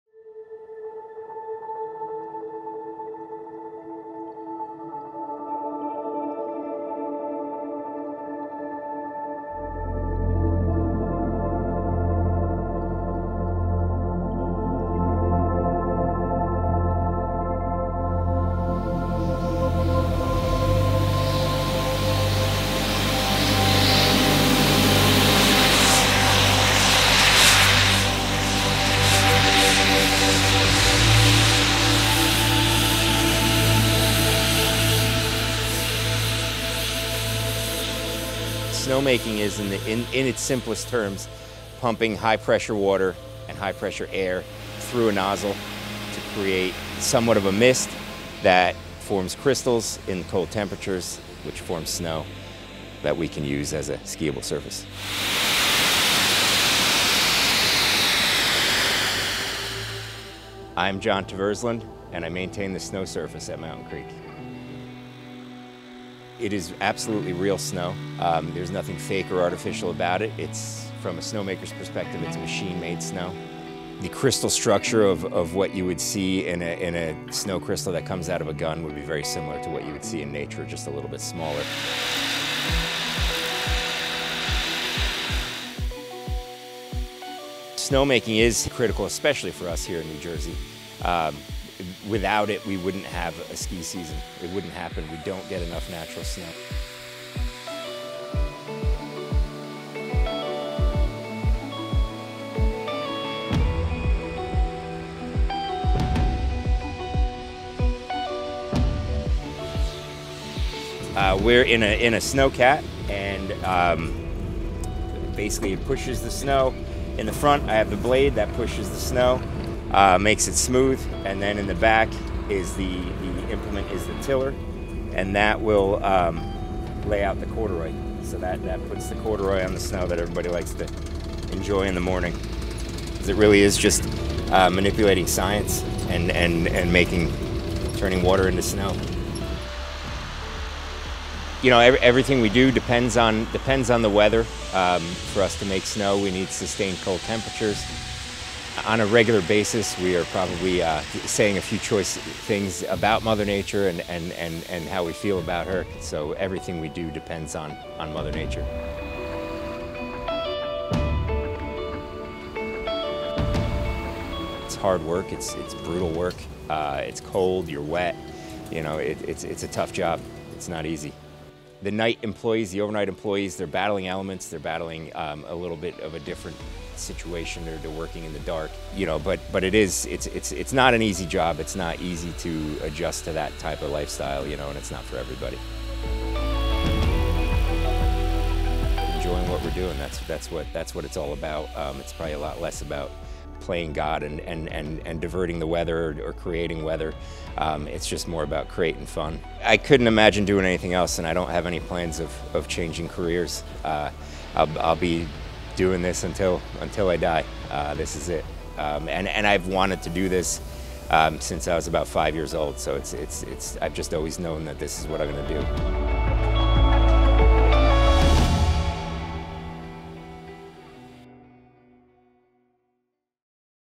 N.J. ski resort relies on snow-making. Here’s how it works
About 180 million gallons of water are used annually at Mountain Creek in Sussex County to create man-made snow. All they need is sustained freezing temperatures and this hearty team of snow-makers. (Video by Andre Malok | NJ Advance Media for NJ.com)